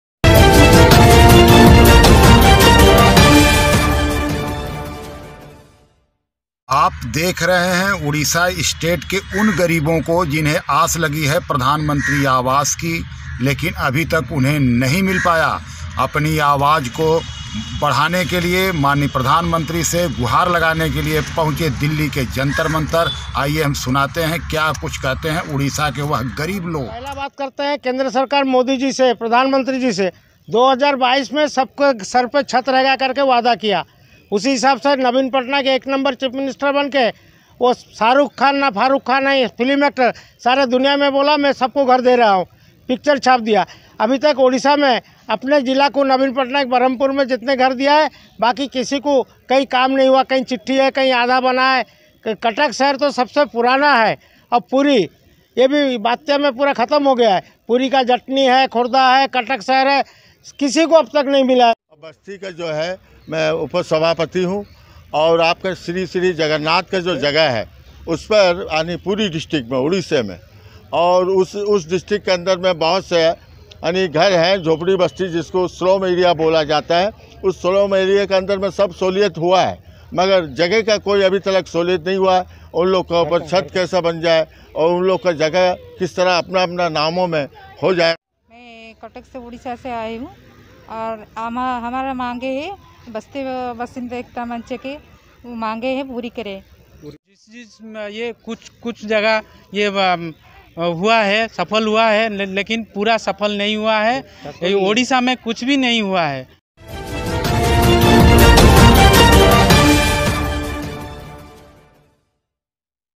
गरीबो ने प्रधान मंत्री आवास मांगने पहुंचे जंतर मंतर-जनसंसद न्यूज।
मोदी जी से गरीबो की पुकार।